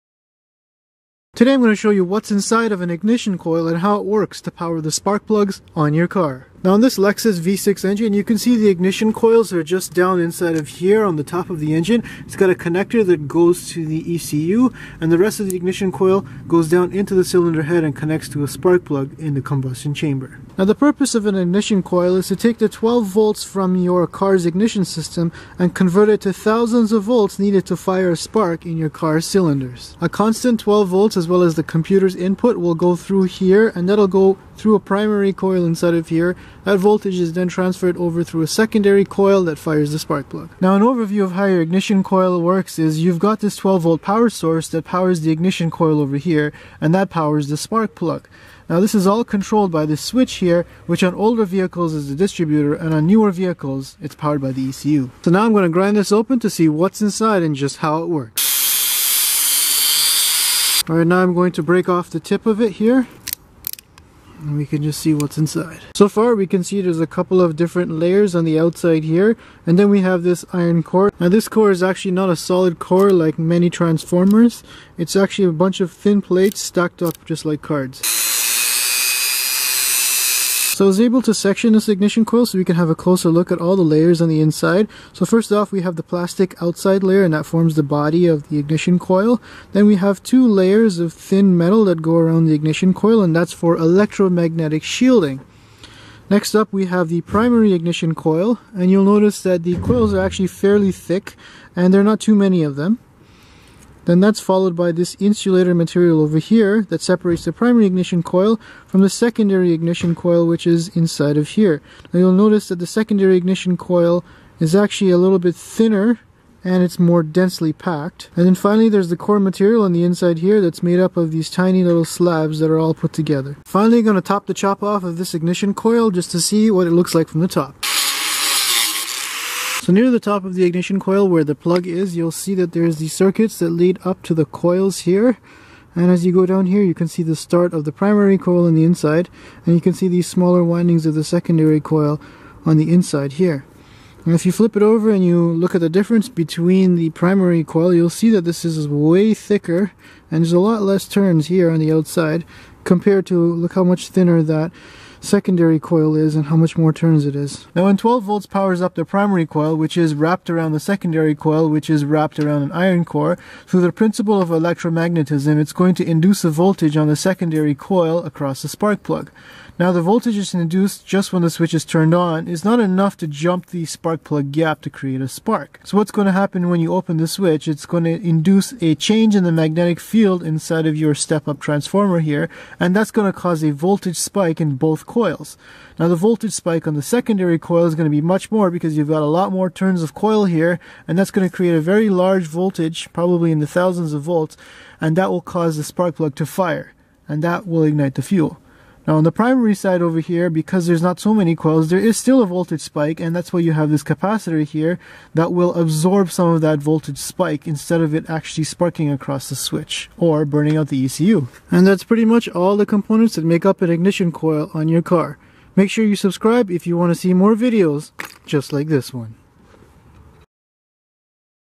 How an Ignition Coil Works
Here's how an ignition coil works on car to fire the spark plug in the combustion chamber.

Most modern vehicles use a coil-over-plug ignition design so the computer can individually program them to maximize efficiency or power.

Spark plugs fire inside the ignition coil to ignite fuel and burn the mixture, pushing the piston down to turn the crank shaft. To fire the spark plug, a rapid high voltage is required for reliable clean combustion. The ignition coil is responsible for increasing the voltage from the car's 12V power system to the thousands of volts needed to fire a spark plug.

The ignition coil is essentially a step up transformer, taking a smaller primary winding wrapped around a more dense secondary coil winding and an iron core. When the circuit switch is opened, the change in the electromagnetic field causes a huge voltage spike that fires across the spark plug tip.

In this video, an ignition coil from a 2001 Toyota Corolla is cut open to show the various layers and components inside, including the shielding, primary and secondary coil windings, insulators as well as the iron core.